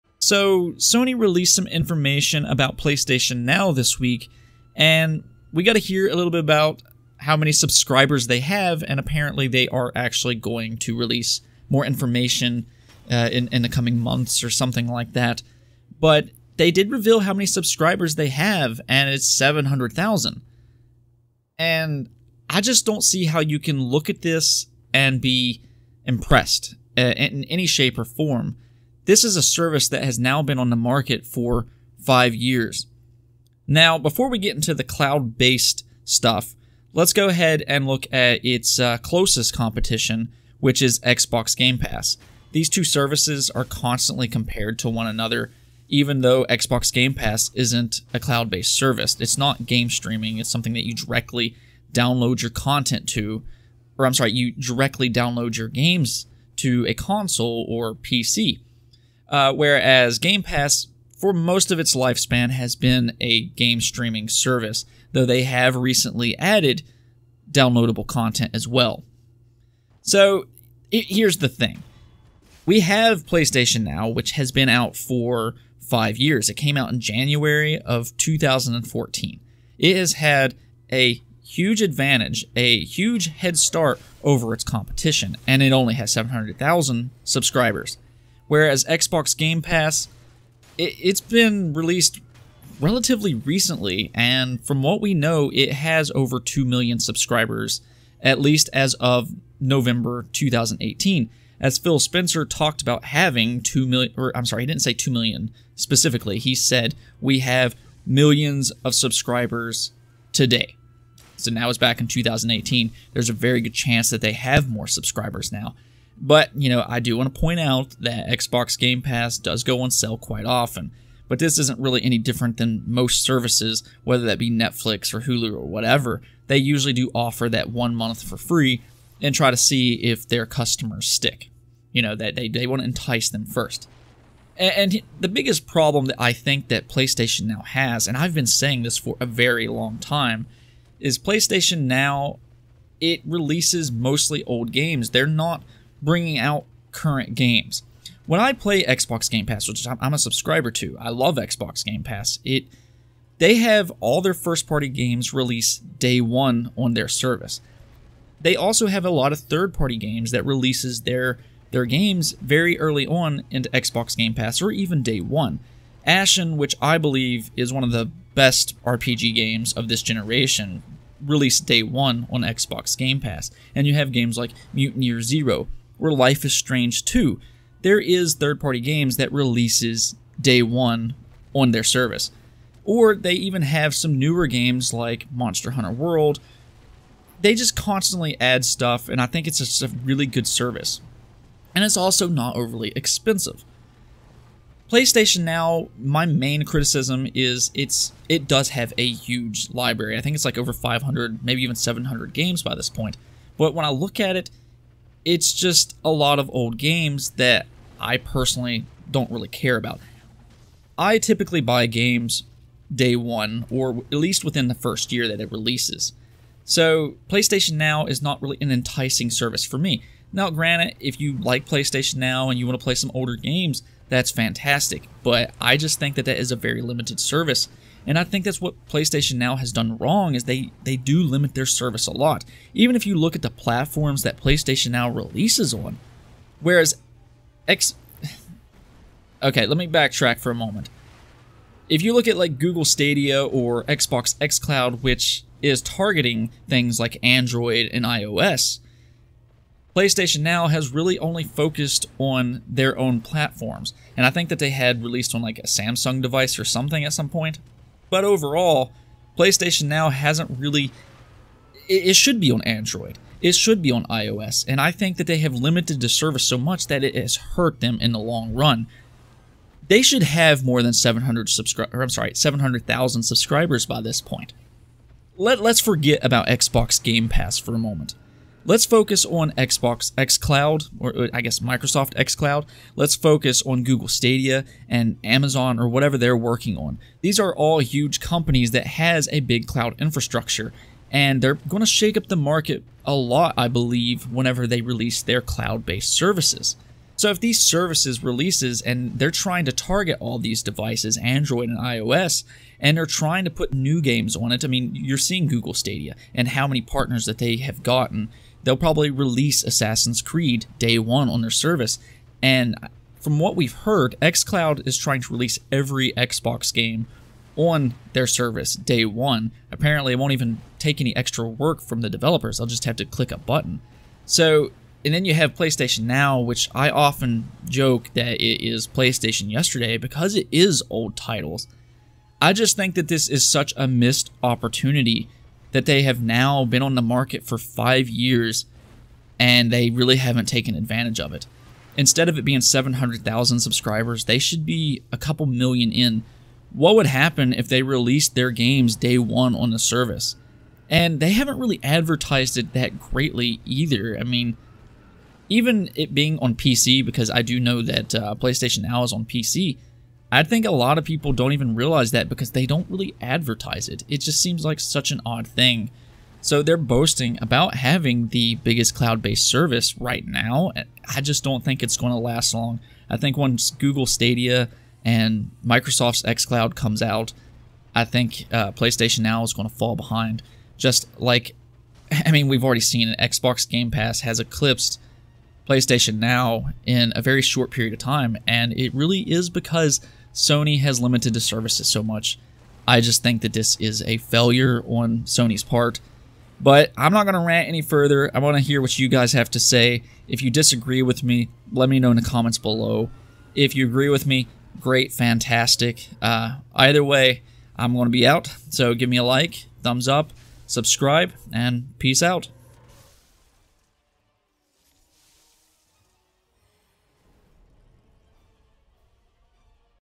PSNow has Failed to Take Advantage of Its Headstart
I personally do not think it is.  Xbox Game Pass has "millions" of subscribers in much less time, which is what PS Now is most often compared to. Let's discuss